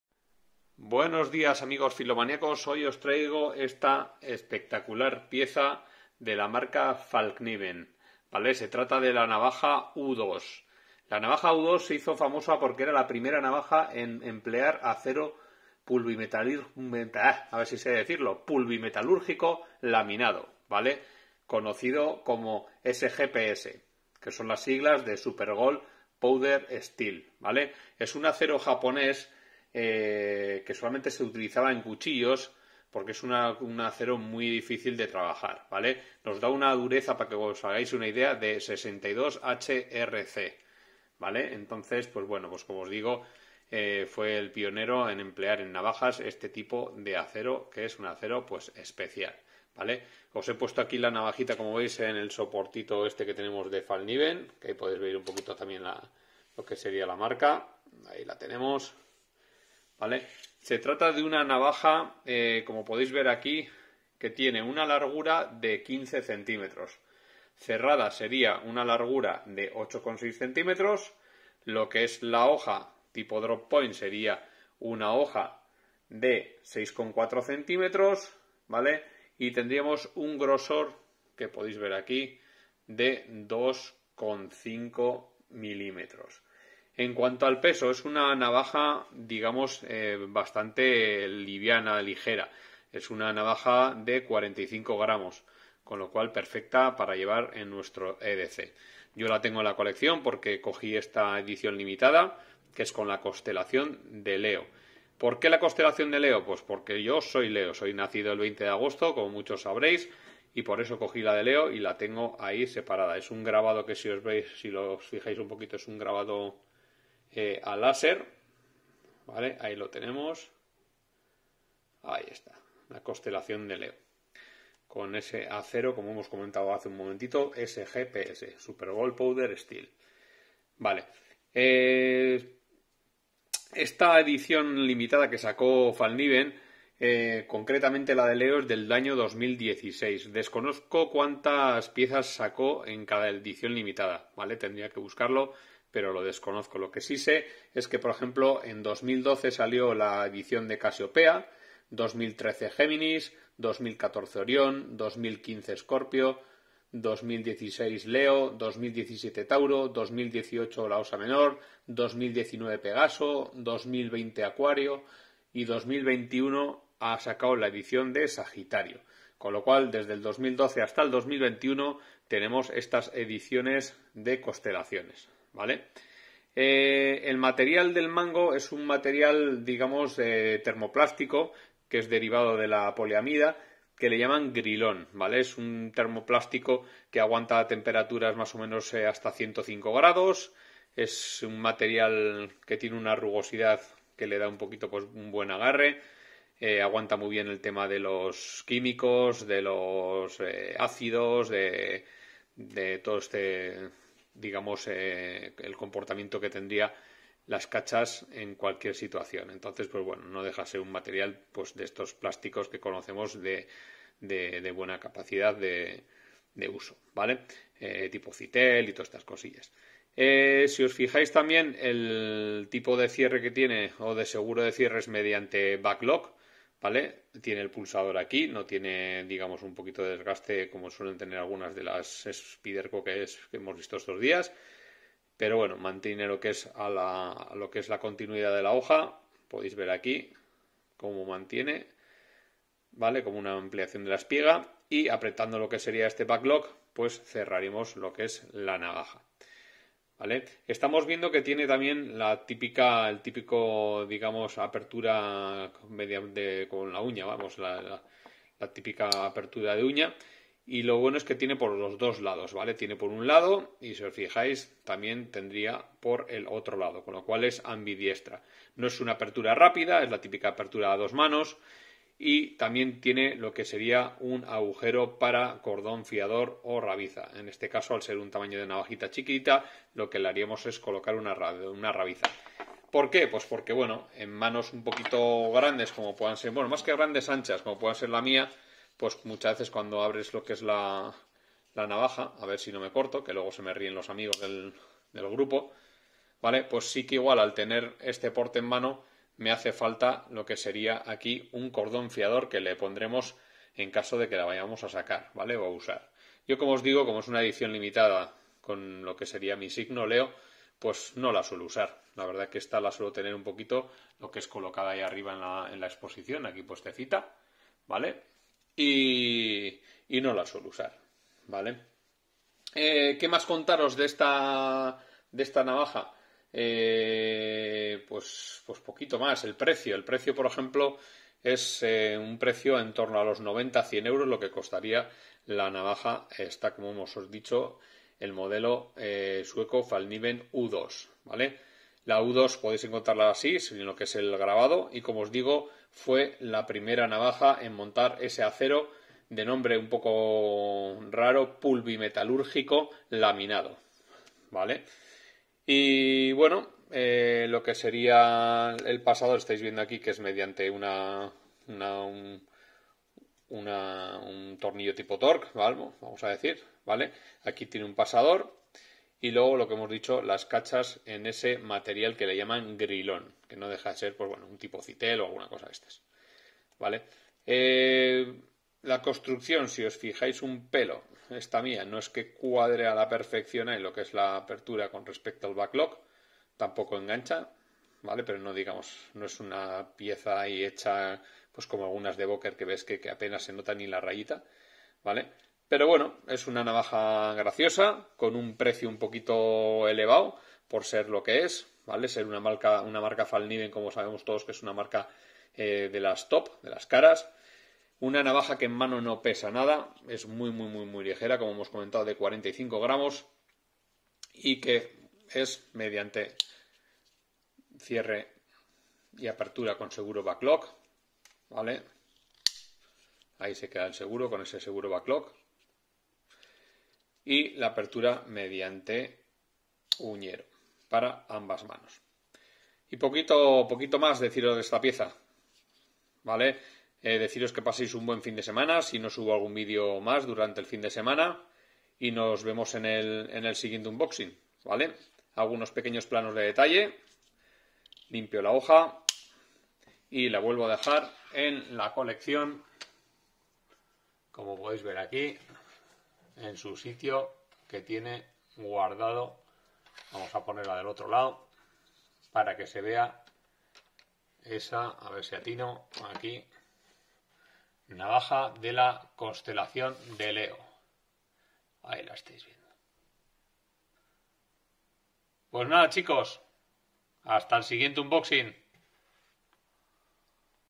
FALLKNIVEN U2 LEO
Hola amigos hoy os traigo una navaja de la casa Fallkniven muy especial. Espero os guste!!!